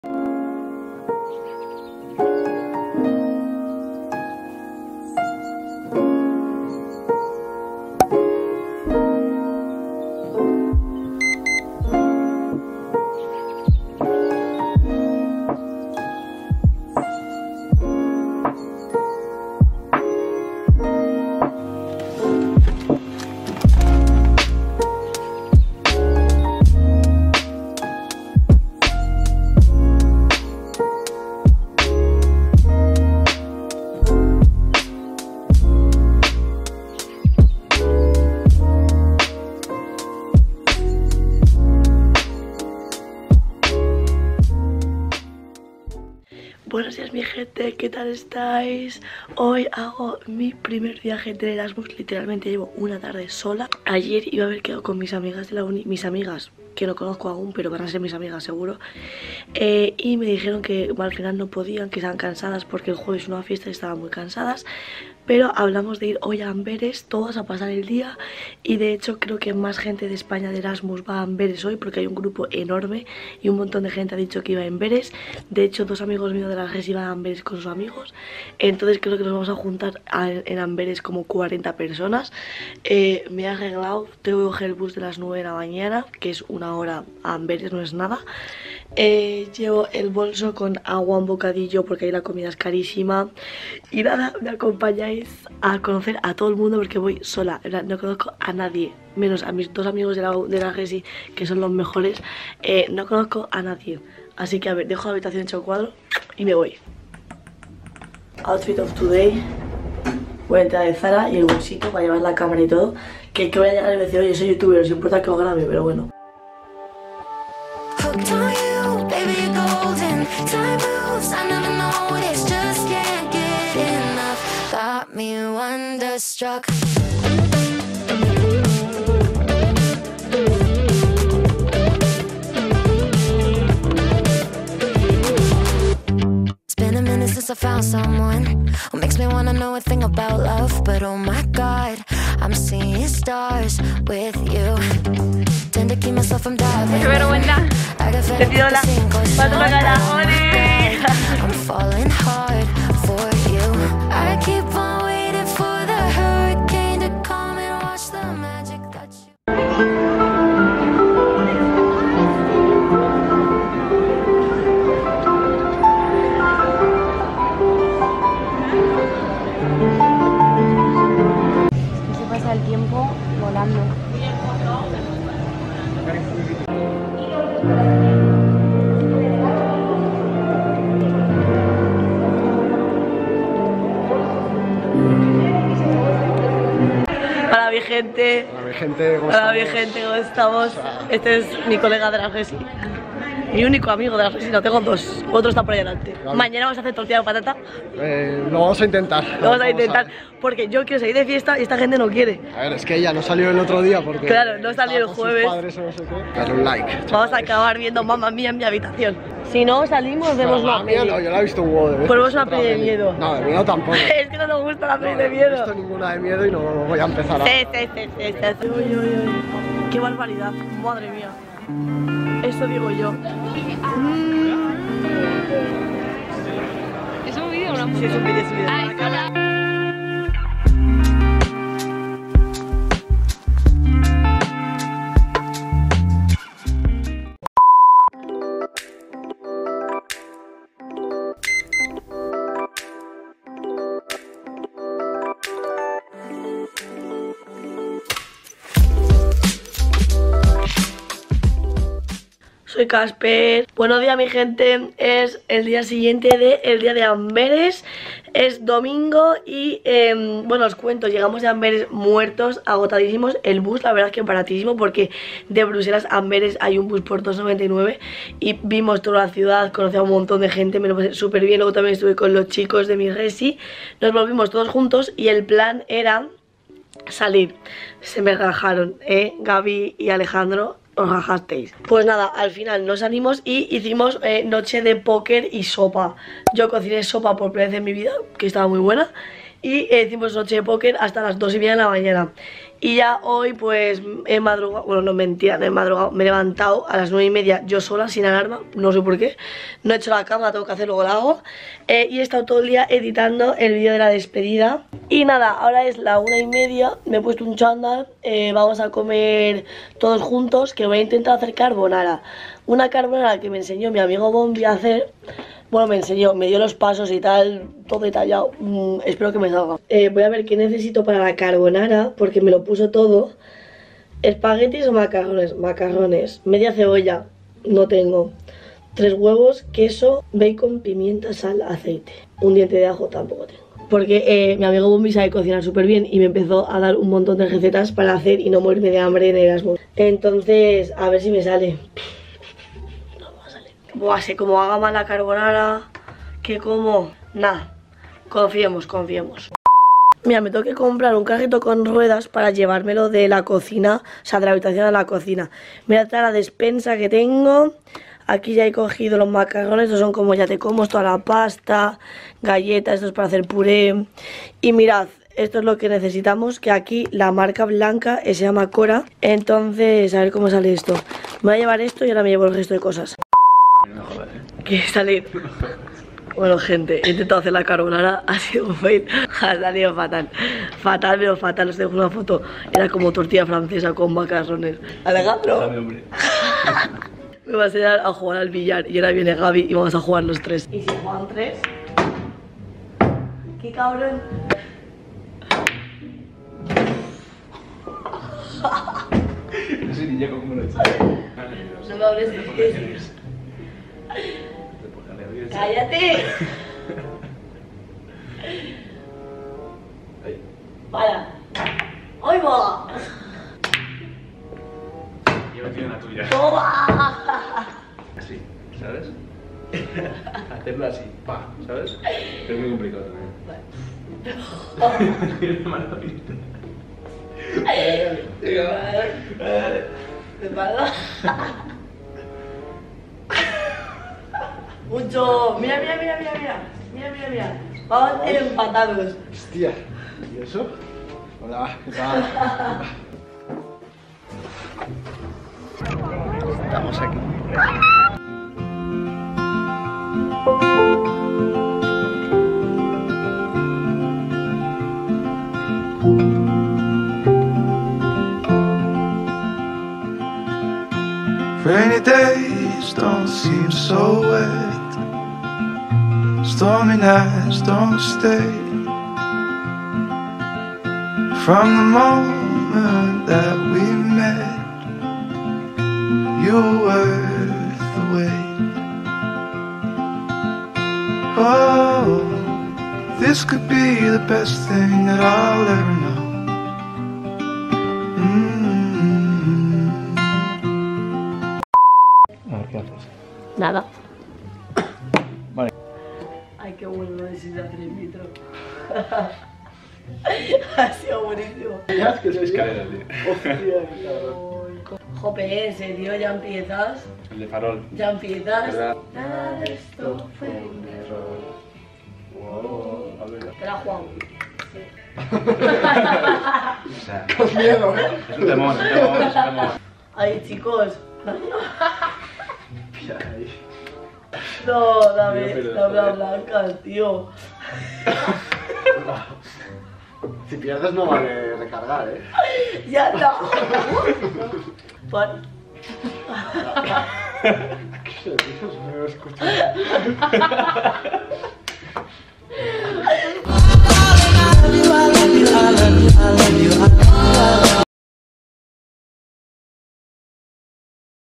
ep. 02 erasmus | viaje a Amberes, mucho póker y atardeceres en la noria 🎡
Hola a todos y a todas❗️ Hoy os traigo el episodio 02 de mi erasmus!! Hice mi primer viaje a Amberes sin conocer a nadie, comimos muchos gofres y nos reímos como nunca!! Además de un poquito de billar y de póker :)
Si os gustan este tipo de vídeos decídmelo en los comentarios⬇️
Espero que os guste un montón!!💛

Nos vemos el domingo que viene a las 15:30 con un nuevo video🎥